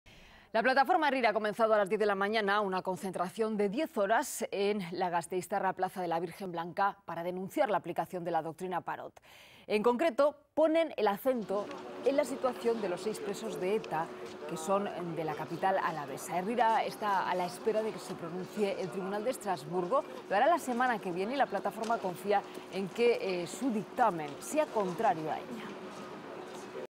Herrira se concentra durante diez horas en protesta por la doctrina Parot
Herrira está a la espera de que se pronuncie el Tribunal de Estrasburgo. Lo hará la semana que viene y la plataforma confía en que su dictamen sea contrario a ella.